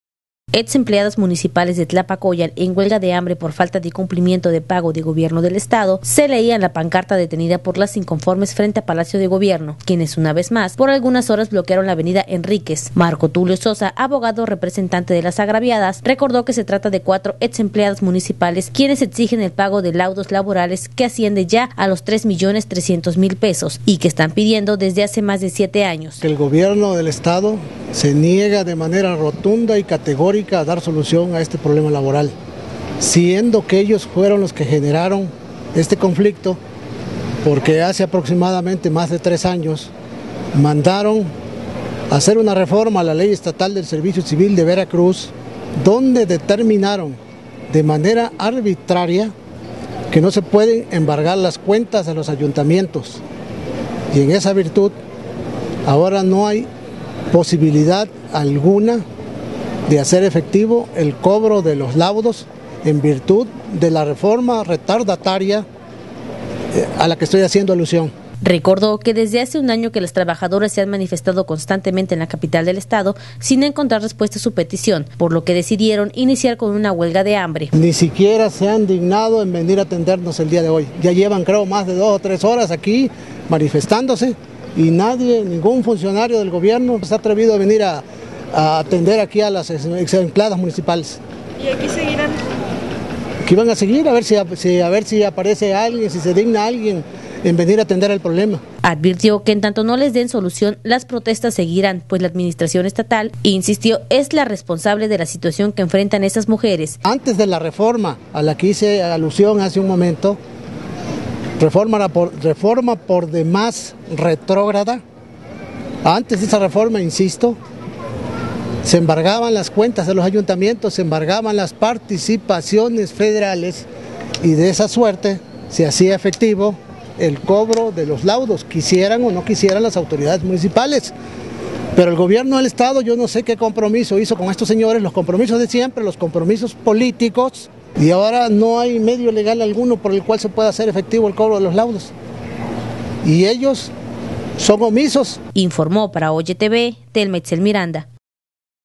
EX EMPLEADAS DE TLAPACOYAN VUELVEN A CERRAR EL CENTRO DE LA CIUDAD
"Exempleadas municipales de Tlapacoyan en huelga de hambre por falta de cumplimiento de pago de Gobierno del Estado", se leía en la pancarta detenida por las inconformes frente a Palacio de Gobierno quienes una vez más, por algunas horas, bloquearon la avenida Enríquez. Marco Tulio Sosa abogado representante de las agraviadas, recordó que se trata de cuatro ex empleadas municipales quienes exigen el pago de laudos laborales que ya asciende a los 3 millones 300 mil pesos, y que están pidiendo desde hace más de siete años.